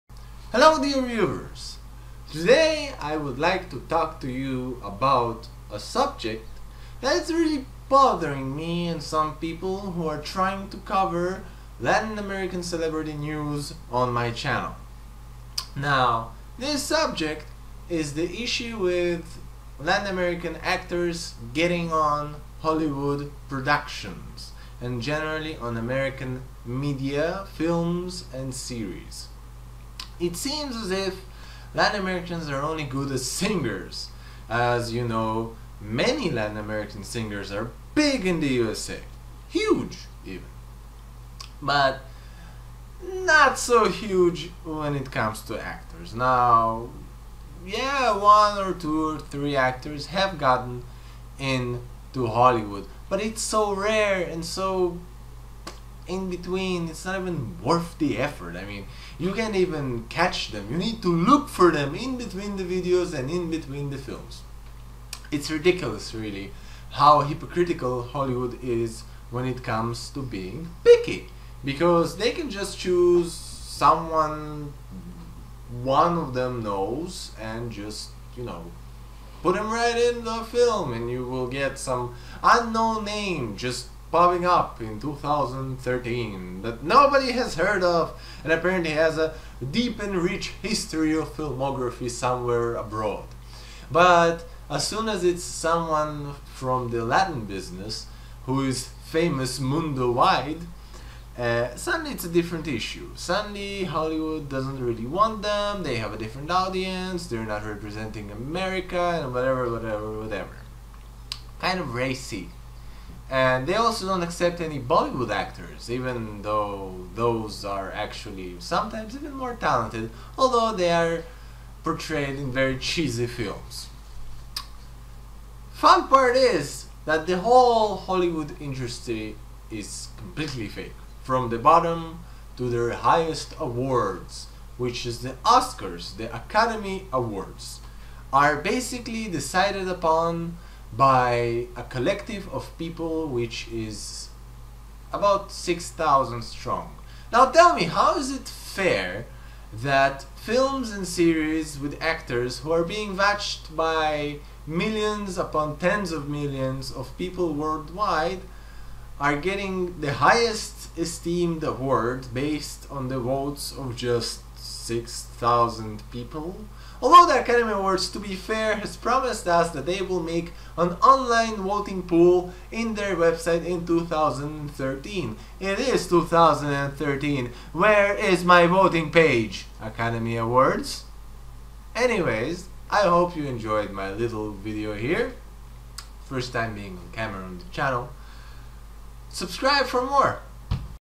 Hollywood is BOGUS
This is Marager speaking again ,and boy am i pissed with this bunch of crap, hollywood better start bringing in some latin american actors!